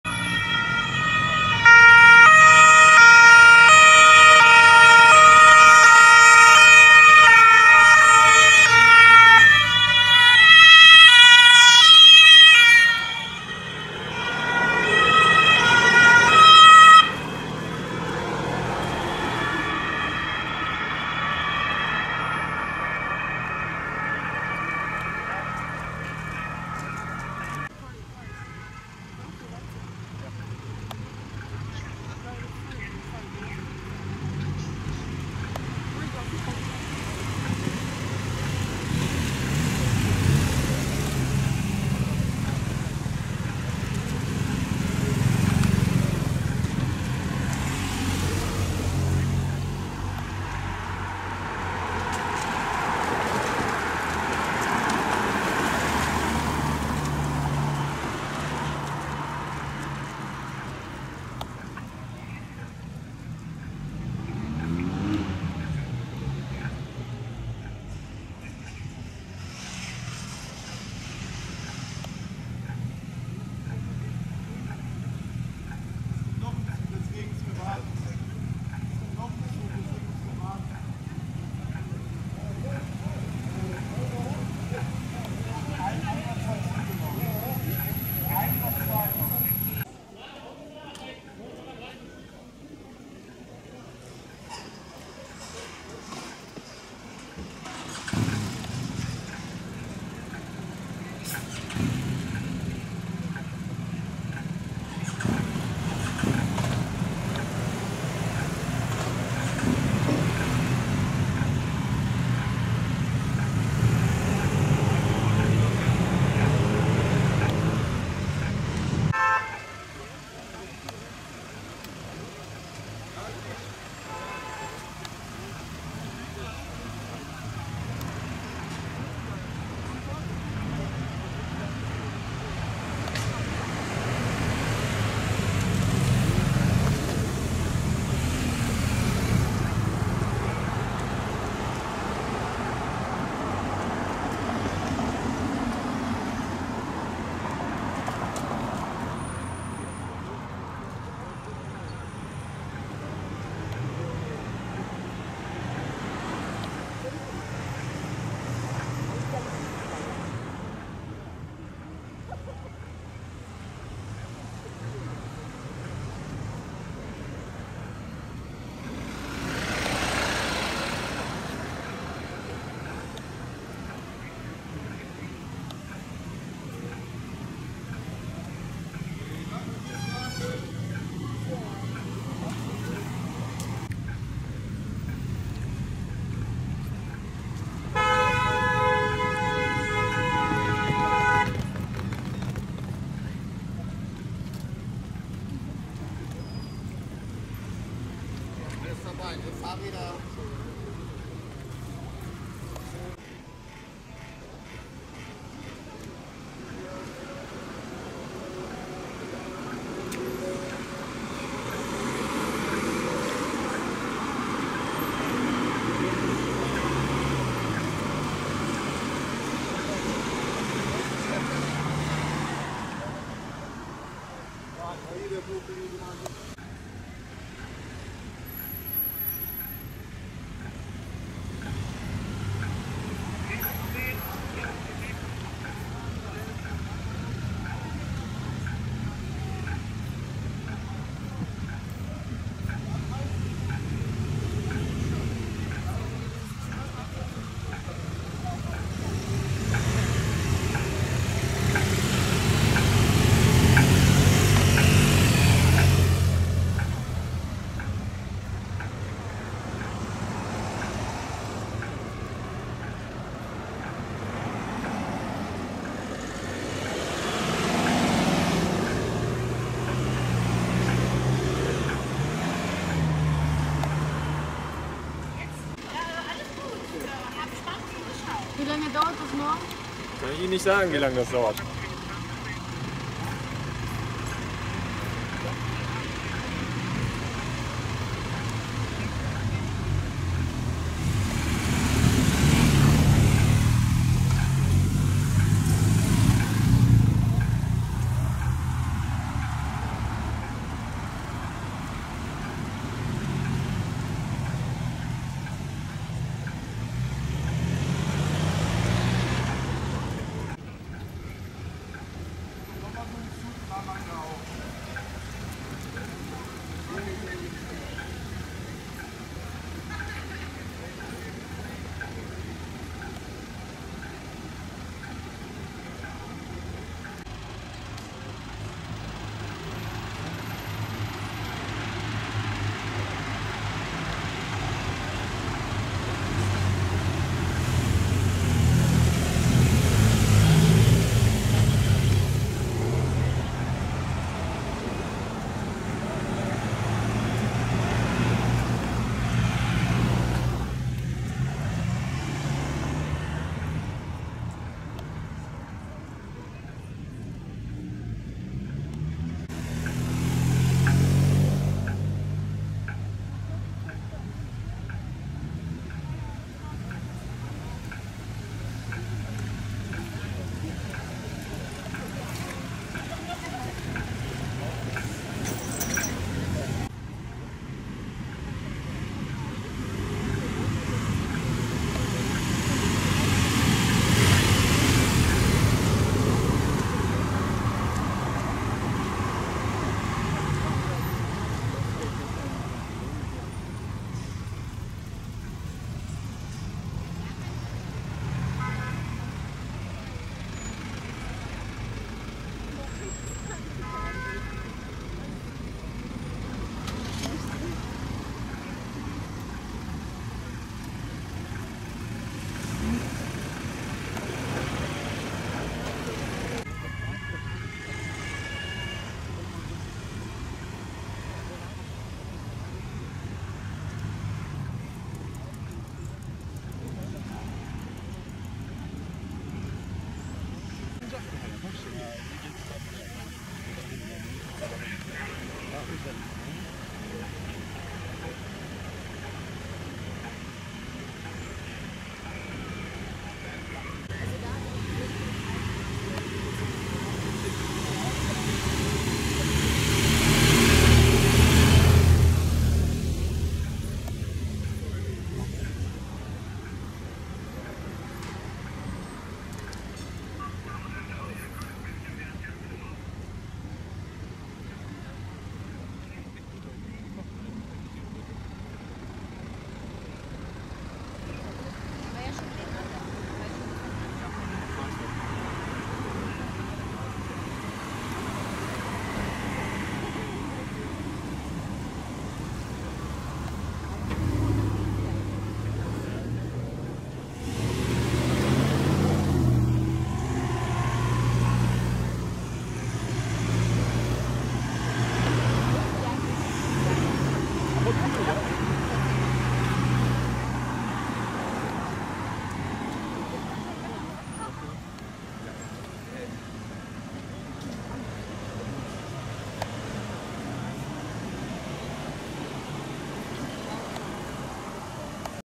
Alle VIP-Eskorten bei G7-Außenministertreffen in Münster am 03.11.22
Alle VIP-Eskorten beim G7-Außenmininistertreffen in Münster am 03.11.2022.